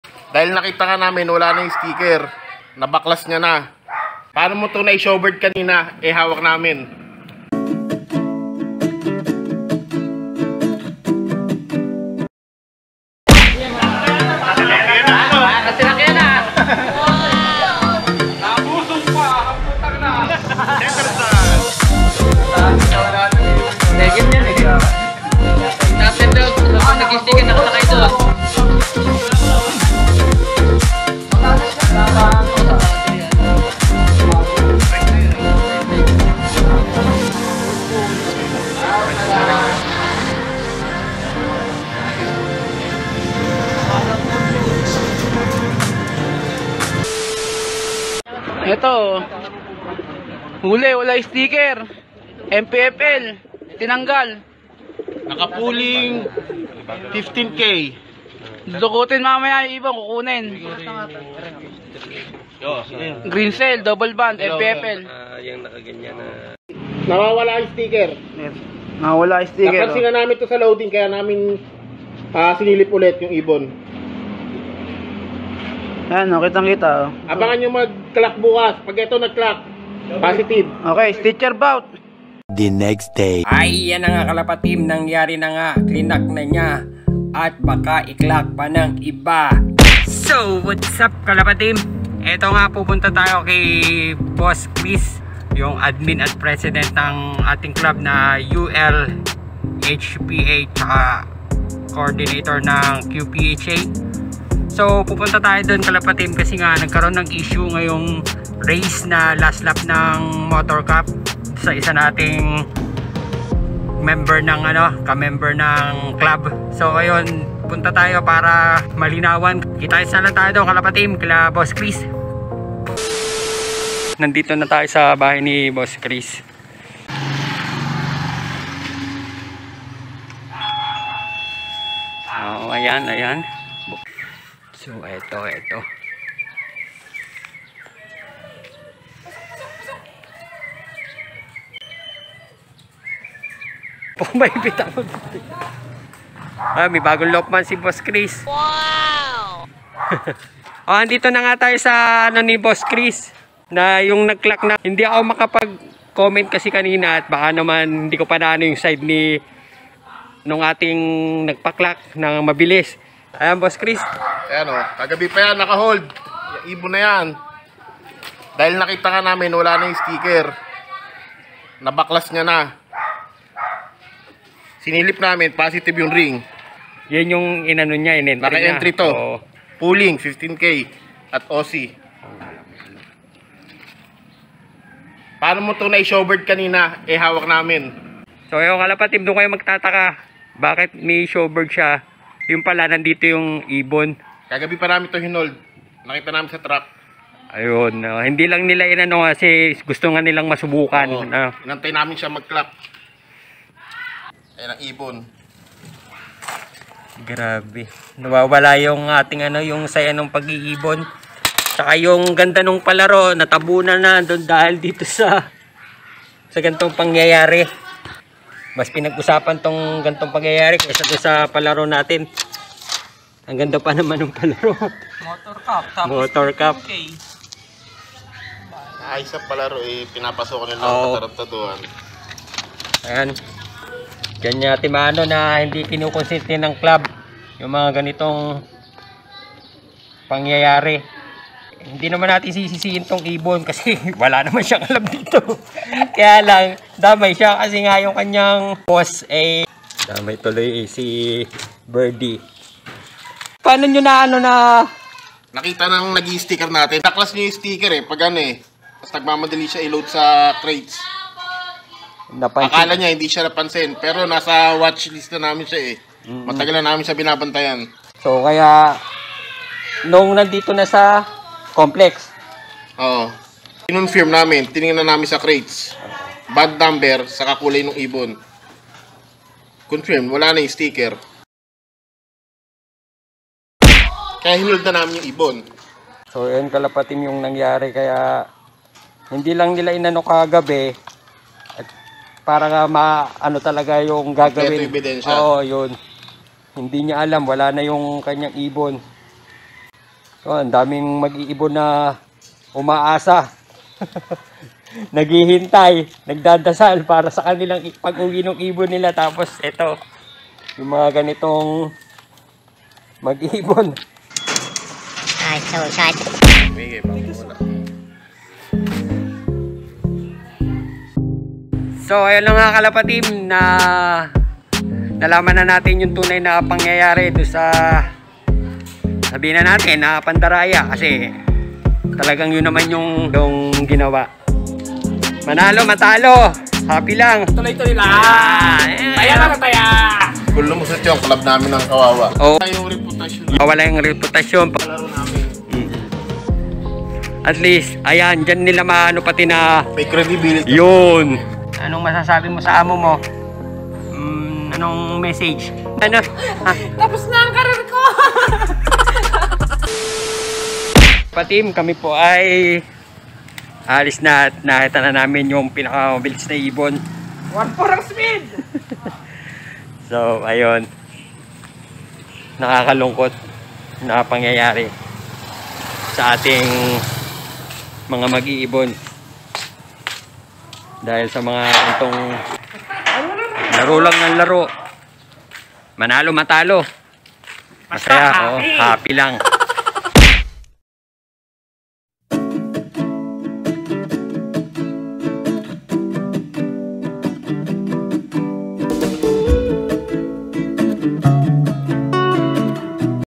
NAHULING PANDARAYA, STICKER NAWAWALA | NAKA HOLD NA IBON PERO NAG CLOCK PA? ANYARE😱 I vlog 50
Reminder: DO NOT SEND HATE TO THE PEOPLE INVOLVED IN THE ISSUE. THE PROBLEM IS ALREADY ACKNOWLEDGED AND WILL BE SOLVED BY THE CLUB OFFICIALS. Thank you!

Hindi ko gustong husgahan agad ang nasabing magiibon, pero hindi rin mapagkakaila na mali ang nagawa niya. Maaaring matindi ang pangangailangan niya sa oras na ito, pero paano naman ang iba na ganun din at patas lumaro? Sana wag na itong maulit, Kalapateam.

Salamat din sa mga ULHPA Club Officials na mabilis umaksyon!

SALAMAT SA SUPORTA AT PANUNUOD MGA KALAPATEAM! KITA-KITS SA SUSUNOD NG VLOG.